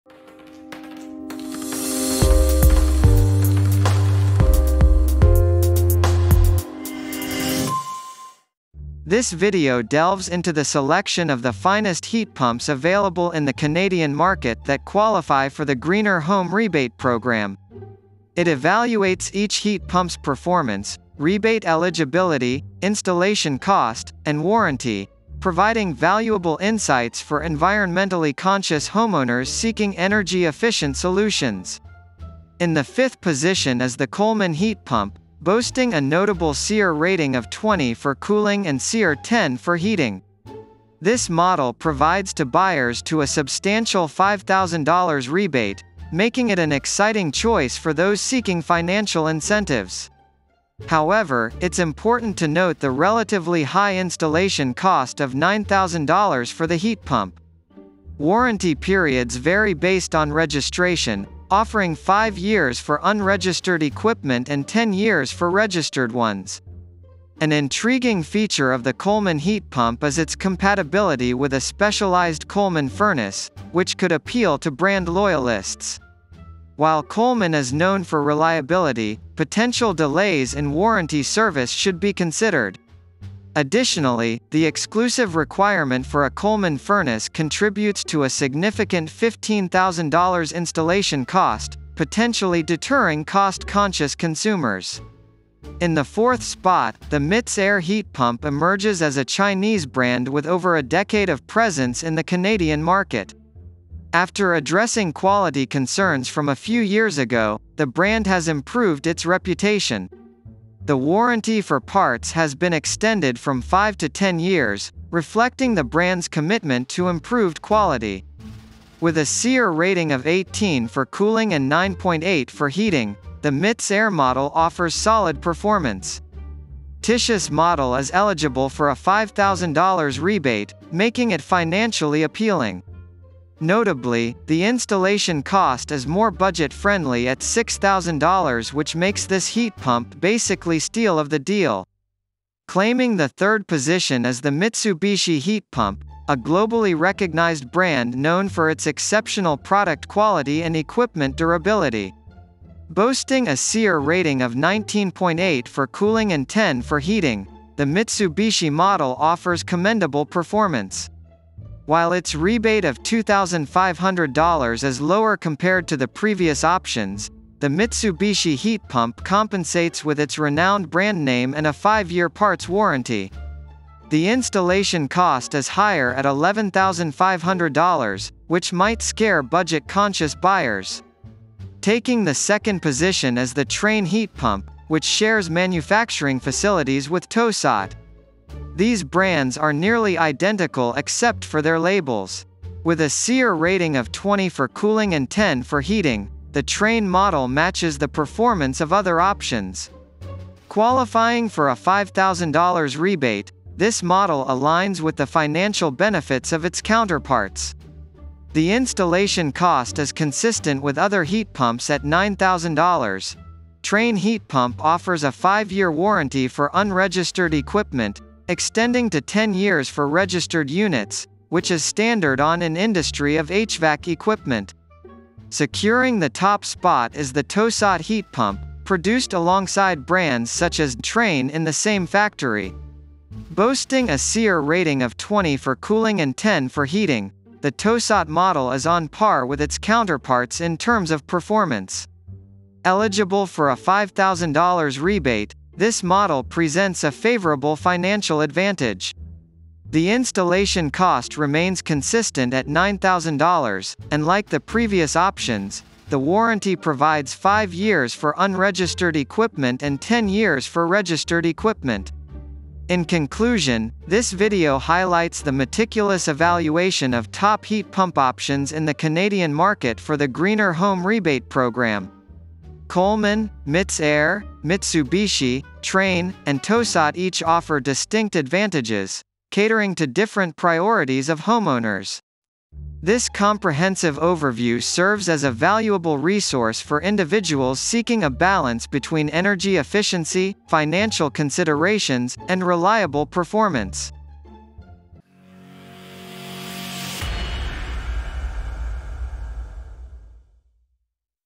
Rating Of the Best Heat Pumps Legible for Greener Home rebate Program!!!
This video delves into the selection of the finest heat pumps available in the Canadian market that qualify for the Greener Home Rebate Program. It evaluates each heat pump's performance, rebate eligibility, installation cost, and warranty, providing valuable insights for environmentally conscious homeowners seeking energy-efficient solutions.